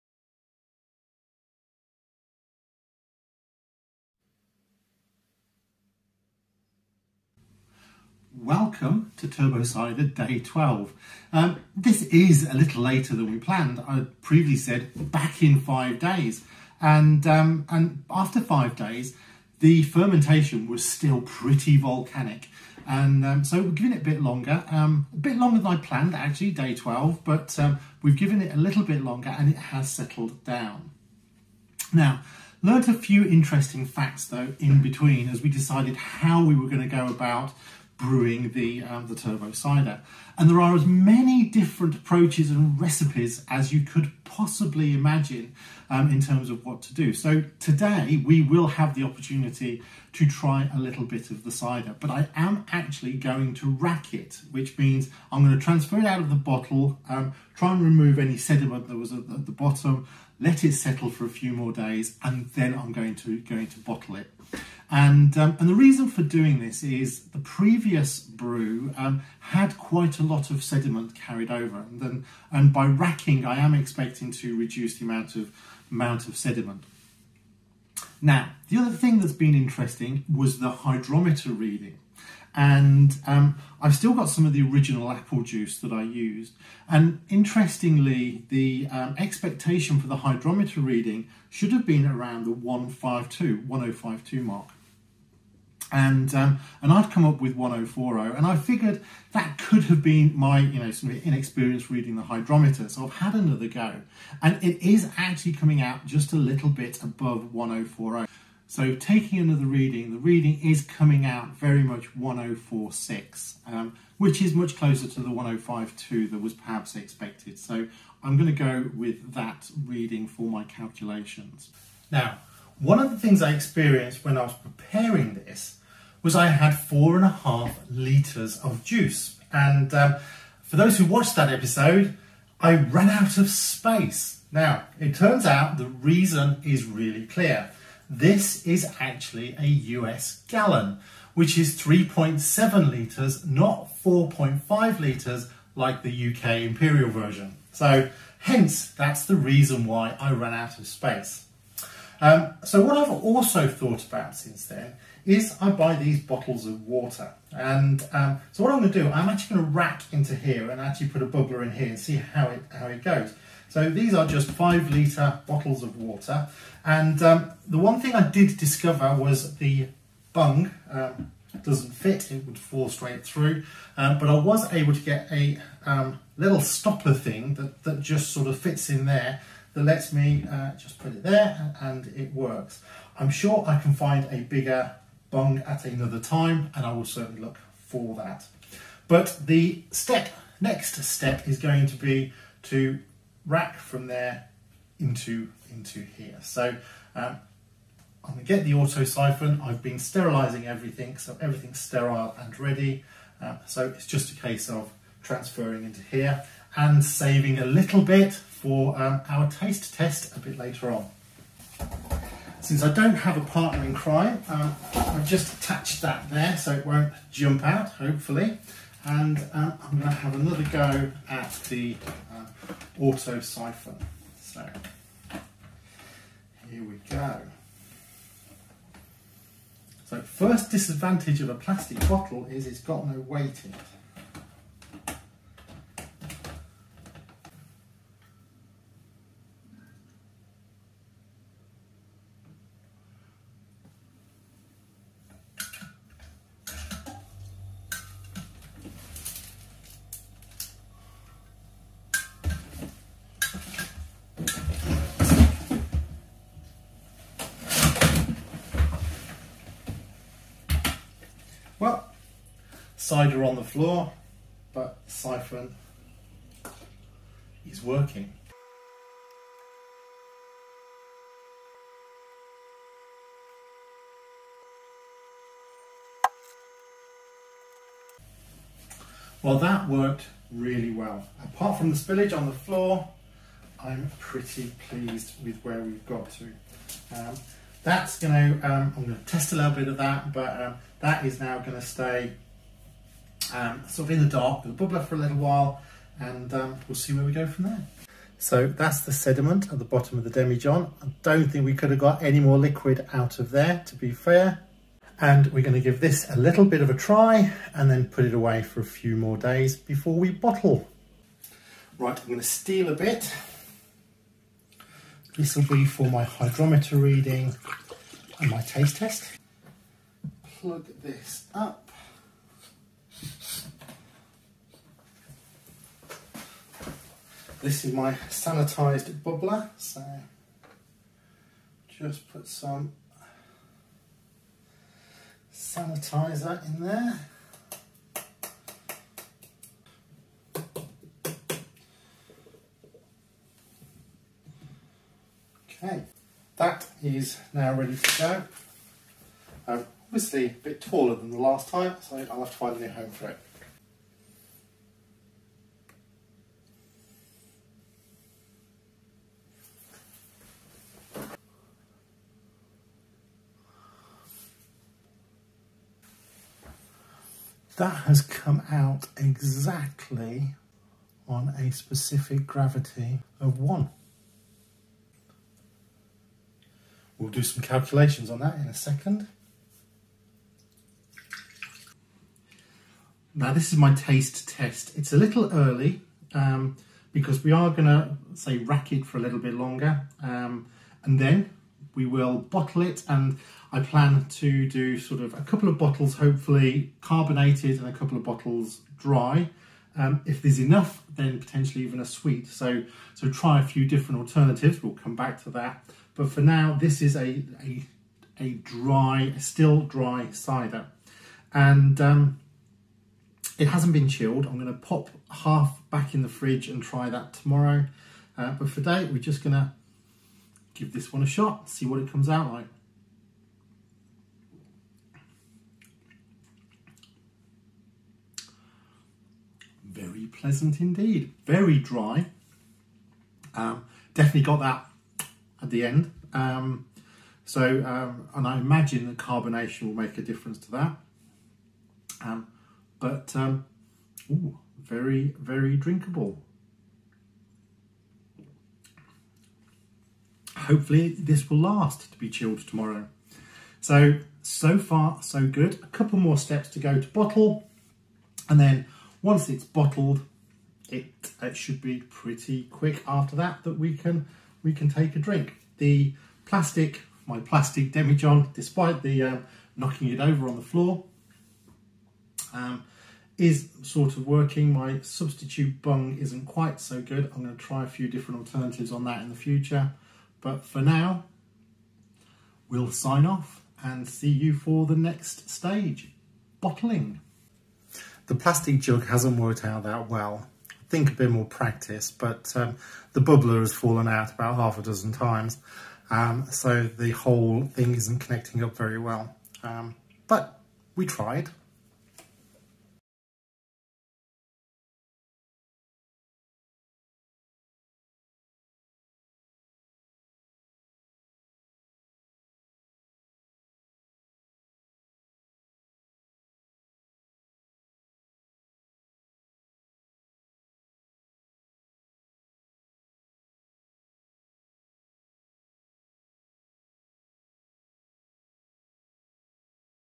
s03e03 Turbo Cider - Racking
Welcome to Brew Number 3: Turbo Cider.  I am very excited about this one, as it promises a quick and tasty result with far less preparation time needed.    Turbo Cider has proved to be an adventure, from discovering problems caused by a difference between measurements for American vs UK Gallons, though to drilling holes in plastic bottle tops to make cheap fermentation vessels.  Bring along a mop for this one, continuing with Part 3 "Racking" as - with good intentions - end up with quite a lot of cider on the floor, rather than in the jar.  What could possibly go wrong with such a simple step?  Tune in to discover!